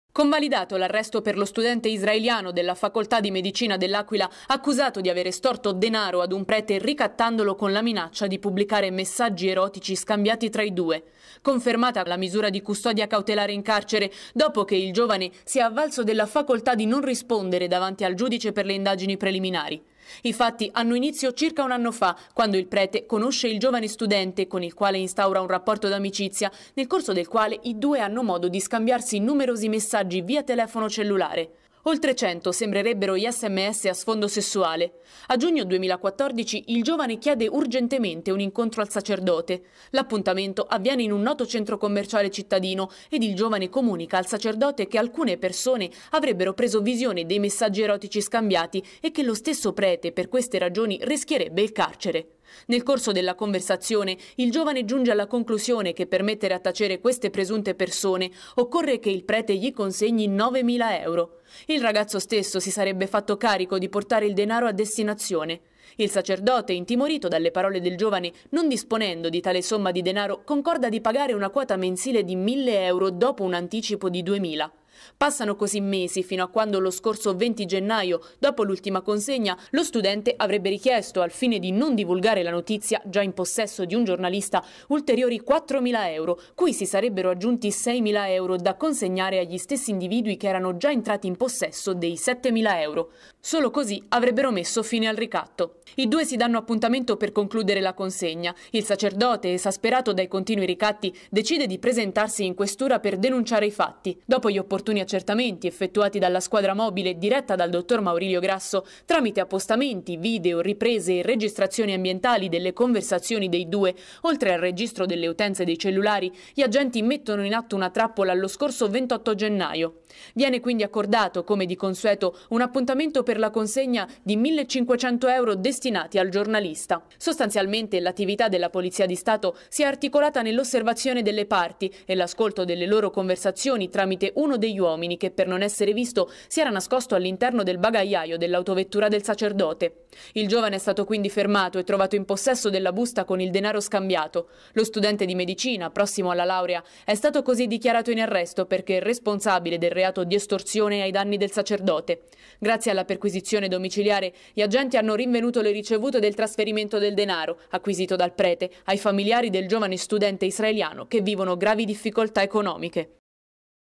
Arrestato per ricatto sessuale a parroco
La denuncia è arrivata dopo la consegna ad uno studente di 7500 euro. Il sacerdote ha così posto fine, assieme alle forze dell'ordine, al ricatto a sfondo sessuale perpetrato attraverso messaggi su Whatsapp